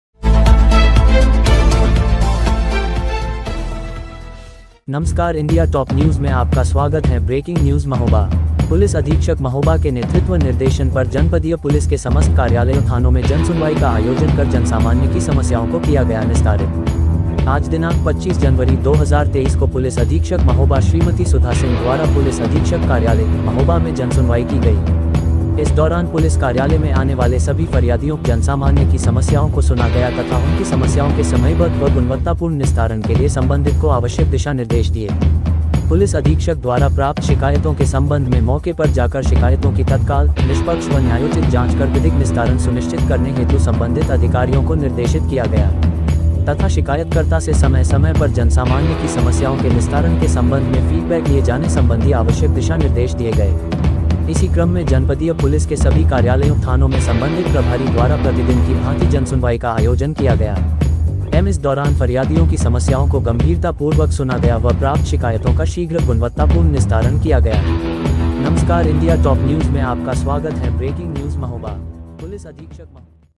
पुलिस अधीक्षक महोबा के नेतृत्व / निर्देशन पर जनपदीय पुलिस के समस्त कार्यालयों/थानों में
*पुलिस अधीक्षक महोबा के नेतृत्व / निर्देशन पर जनपदीय पुलिस के समस्त कार्यालयों/थानों में जनसुनवाई का आयोजन कर जनसामान्य की समस्याओं को किया गया निस्तारित-*

          आज दिनांक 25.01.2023 को पुलिस अधीक्षक महोबा श्रीमती सुधा सिंह द्वारा पुलिस अधीक्षक कार्यालय, महोबा में जनसुनवाई की गई, इस दौरान पुलिस कार्यालय में आने वाले सभी फरियादियों/जनसामान्य की समस्याओं को सुना गया तथा उनकी समस्याओं के समयबद्ध व गुणवत्तापूर्ण निस्तारण के लिए सम्बन्धित को आवश्यक दिशा-निर्देश दिए, पुलिस अधीक्षक द्वारा प्राप्त शिकायतों के सम्बन्ध में मौके पर जाकर शिकायतों की तत्काल, निष्पक्ष व न्यायोचित जांच कर विधिक निस्तारण सुनिश्चित करने हेतु संबंधित अधिकारियों को निर्देशित किया गया तथा शिकायतकर्ता से समय-समय पर जनसामान्य की समस्याओं के निस्तारण के सम्बन्ध में फीडबैक लिये जाने सम्बन्धी आवश्यक दिशा-निर्देश दिये गये।
         इसी क्रम में जनपदीय पुलिस के सभी कार्यालयों/थानों में सम्बन्धित प्रभारी द्वारा प्रतिदिन की भांति जनसुनवाई का आयोजन किया गया, इस दौरान फरियादियों की समस्याओं को गम्भीरता पूर्वक सुना गया व प्राप्त शिकायतों का शीघ्र/गुणवत्तापूर्ण निस्तारण किया गया ।